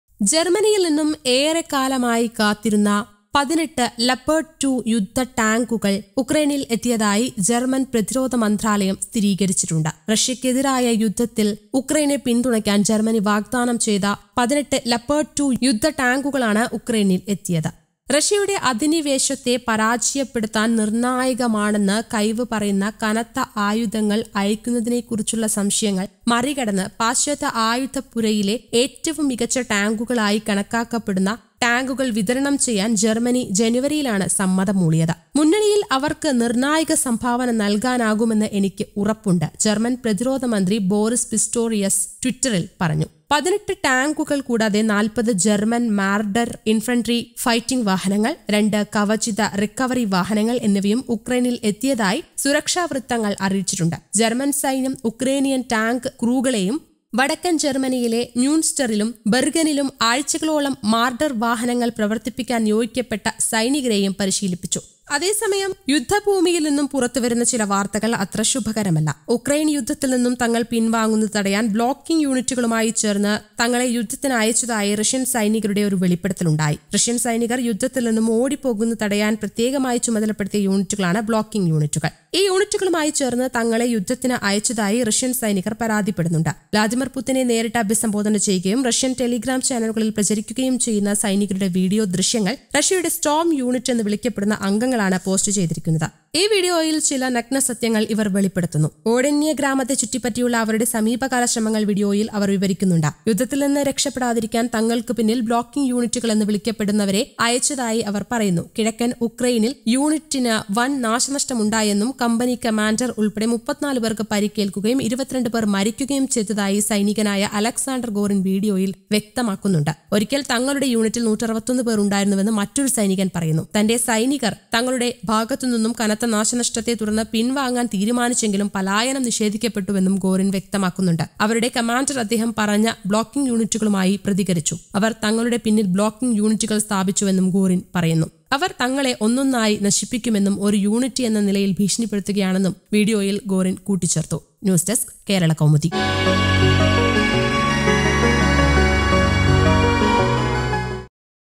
18 'ലെപ്പാർഡ് 2' യുദ്ധടാങ്കുകളെത്തി, ഇനി തീ യുദ്ധം | Leopard 2 Tank
ജർമ്മനിയിൽ നിന്നും ഏറെ കാലമായി കാത്തിരുന്ന 18 ലെപ്പാർഡ് 2 യുദ്ധ ടാങ്കുകൾ യുക്രൈനിൽ എത്തിയതായി ജർമ്മൻ പ്രതിരോധ മന്ത്രാലയം സ്ഥിരീകരിച്ചു